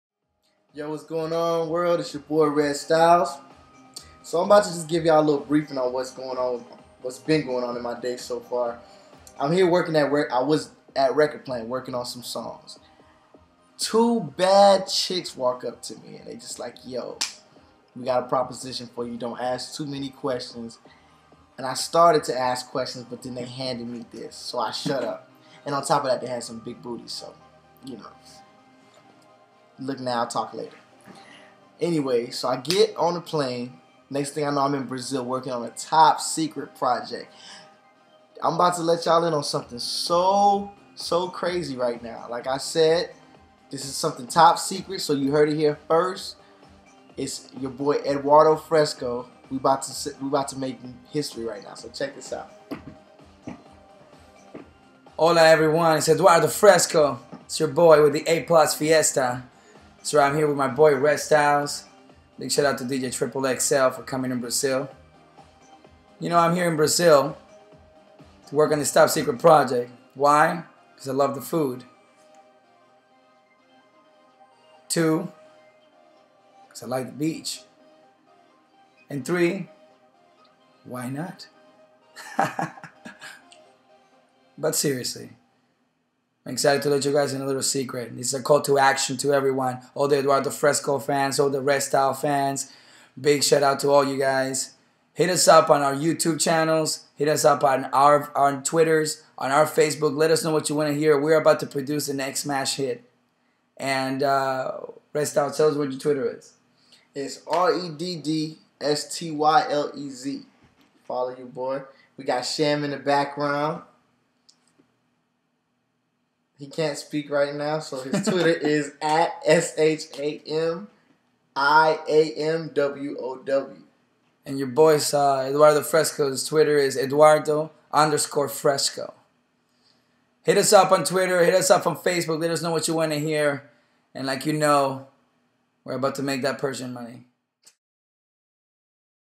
Freslog #2
Here with Redd Stylez, Sham, and DJ Tripple XL in Brazil! Hit us up here: